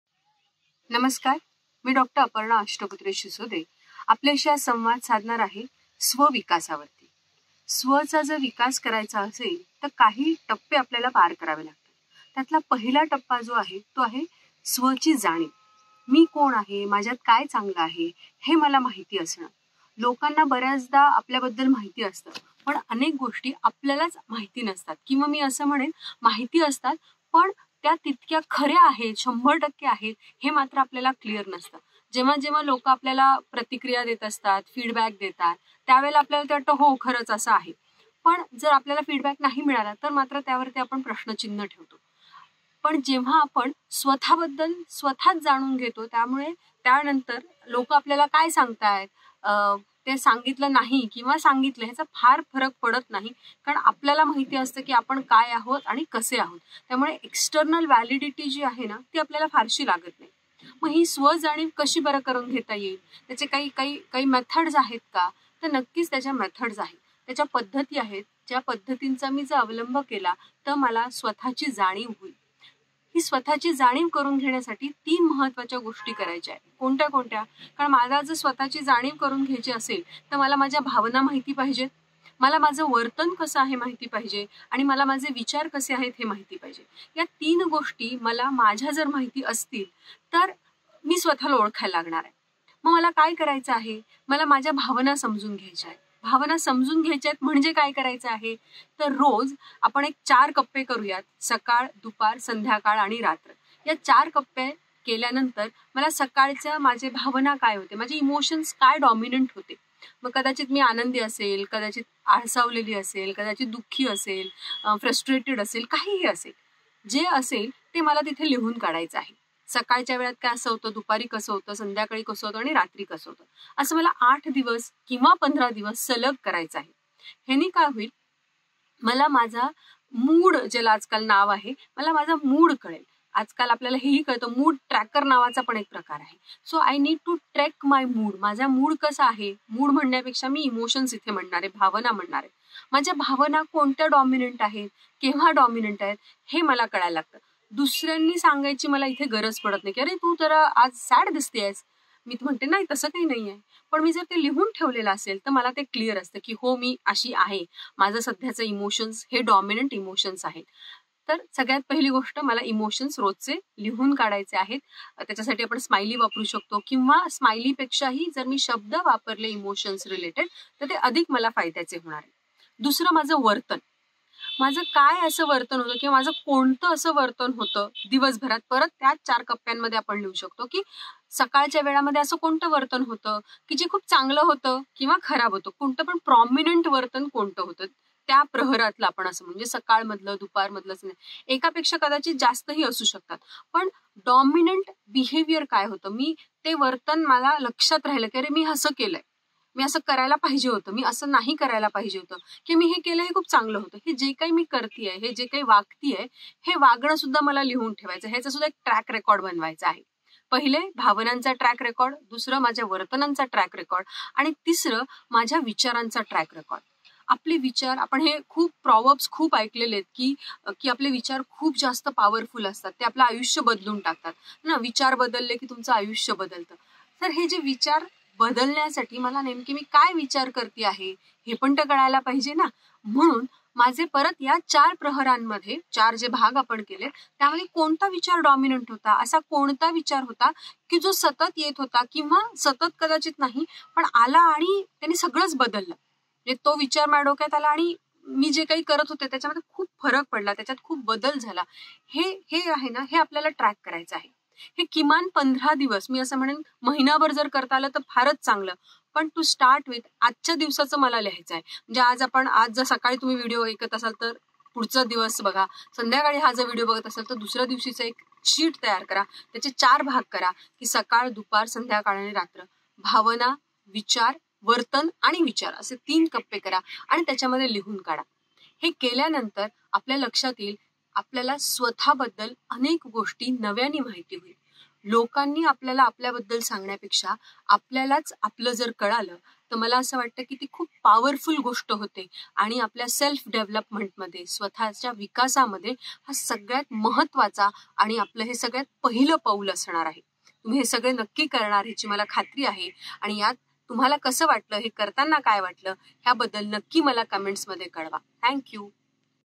Self Development | स्वत:चा विकास
Setting goals and having a clear sense of purpose is critical to achieving success in life. However, it's not enough to simply have goals; you must also take them seriously and commit to them fully. Without this level of focus and commitment, it's easy to lose sight of your goals and get distracted by the many competing demands of daily life.

About me-
I am Dr. Aparna Ashtaputre-Sisode, Professor and Head of Psychology department at Dr. Babasaheb Ambedkar Marathwada University,  Chh.Sambhajinagar, Maharashtra.